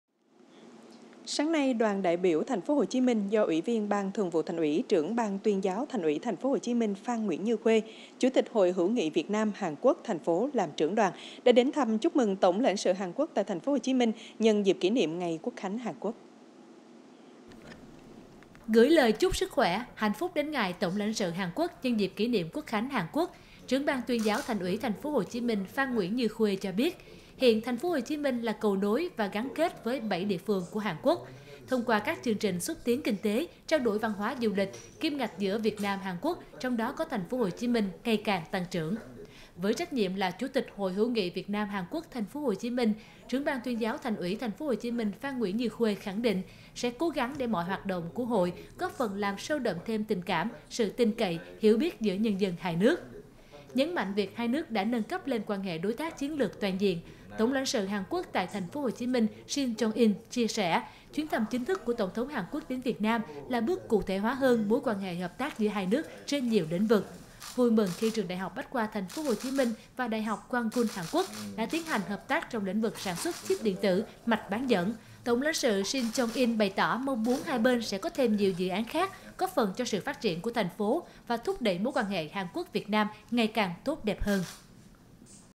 TP.HCM CHÚC MỪNG QUỐC KHÁNH HÀN QUỐC
Đoàn đại biểu TPHCM do Ủy viên Ban Thường vụ Thành ủy, Trưởng Ban Tuyên giáo Thành ủy TP.HCM Phan Nguyễn Như Khuê, Chủ tịch Hội hữu nghị Việt Nam - Hàn Quốc làm trưởng đoàn, đã đến thăm, chúc mừng Tổng Lãnh sự Hàn Quốc tại TPHCM, nhân dịp kỷ niệm ngày Quốc khánh Hàn Quốc.